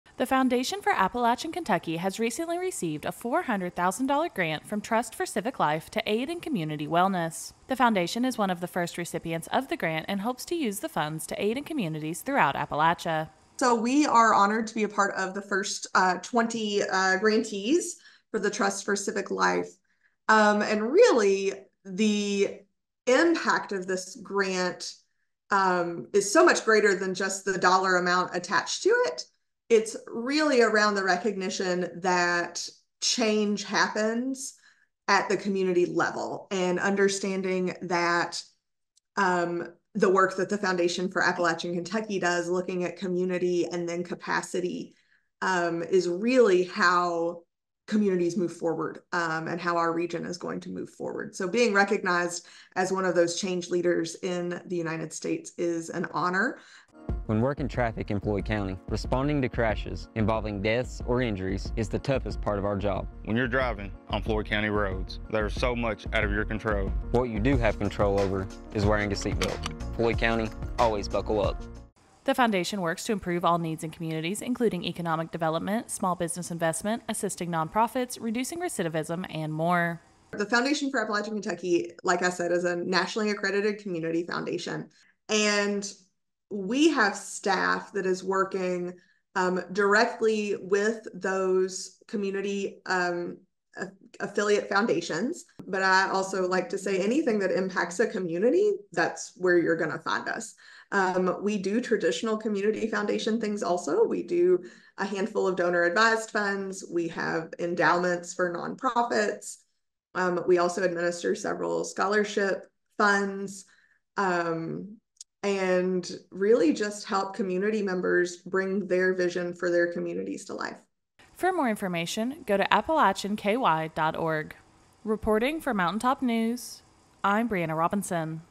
The Foundation for Appalachian Kentucky gets a Grant to Help Community Wellness
The Foundation for Appalachian Kentucky will has received a grant to aid in community wellness.